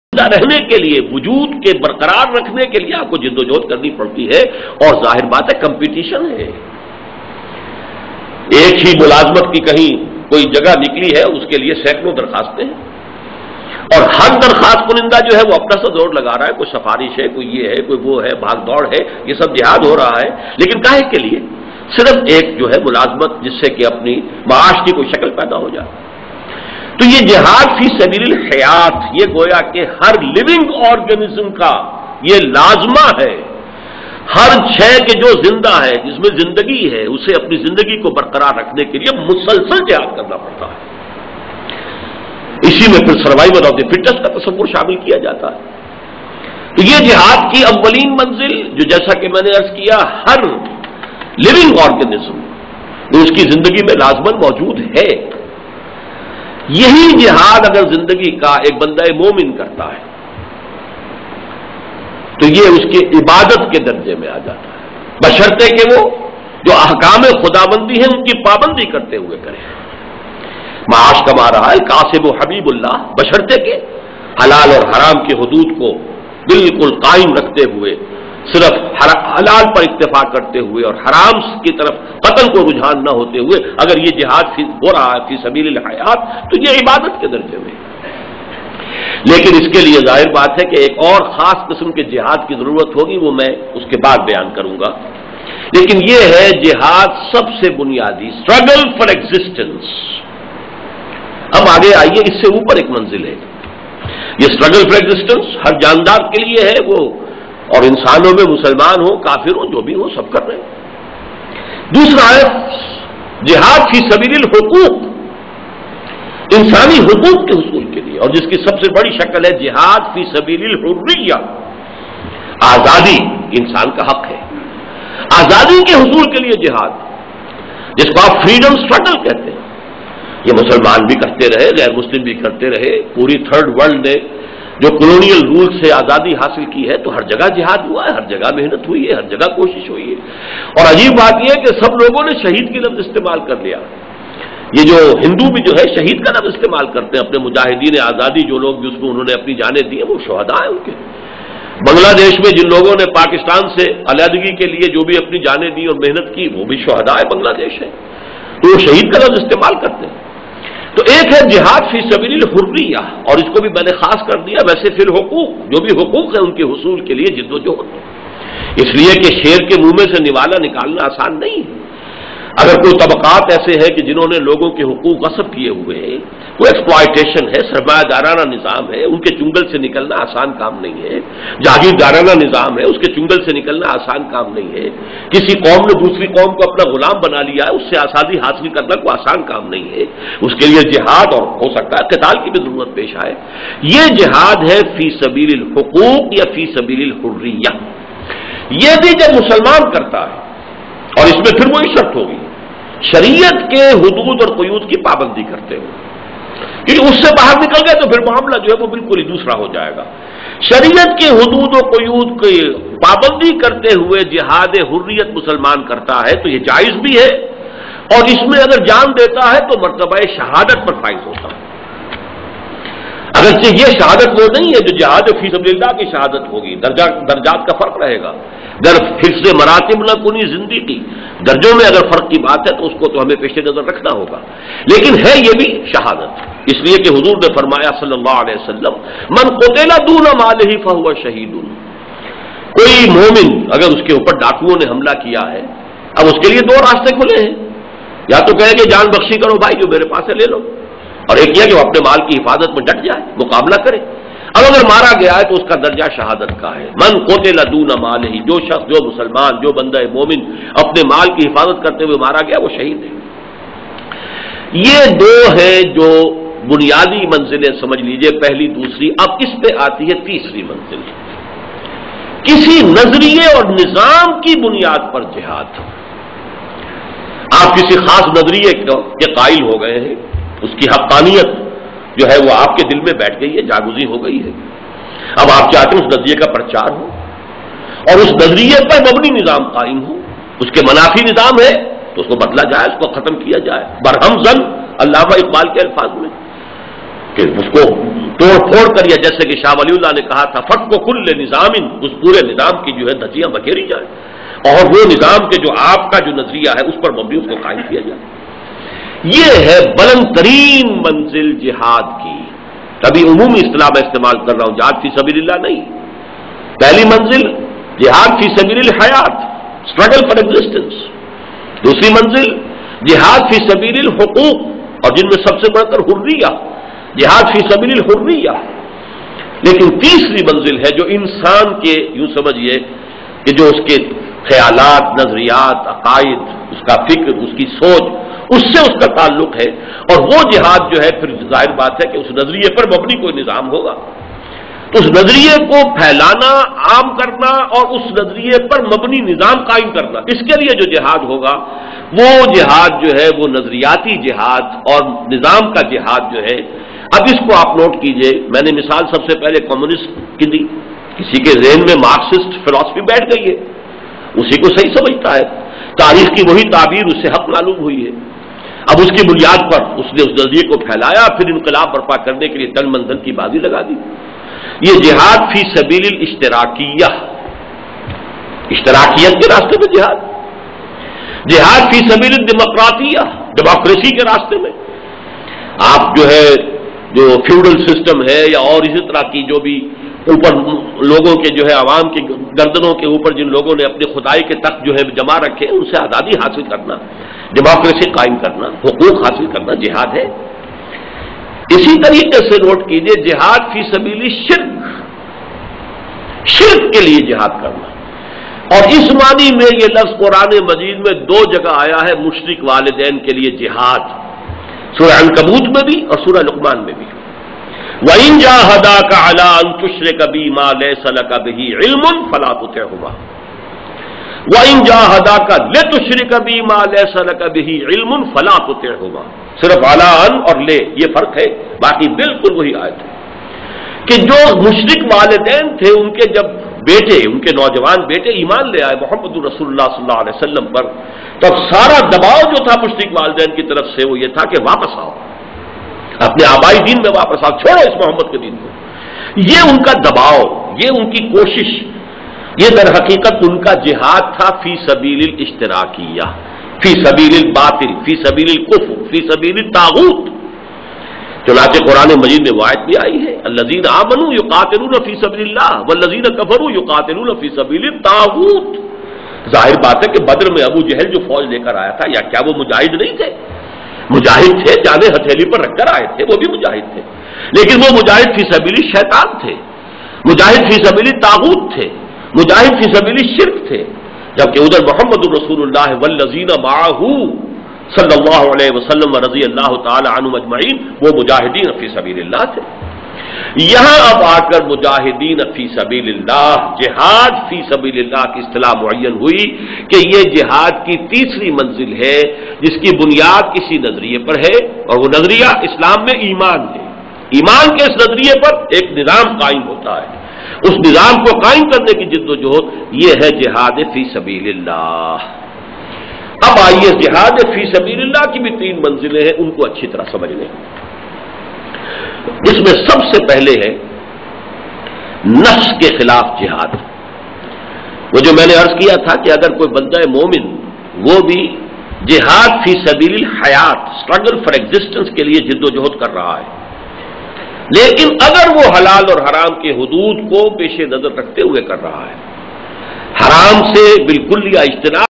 Jihad Fee Sabeel-Lillah 1-5 (Urdu)
Jihad Fee Sabeel-lillah - To strive and struggle in the way of Allah. Explained by Dr. Israr Ahmed (An Islamic Schollar).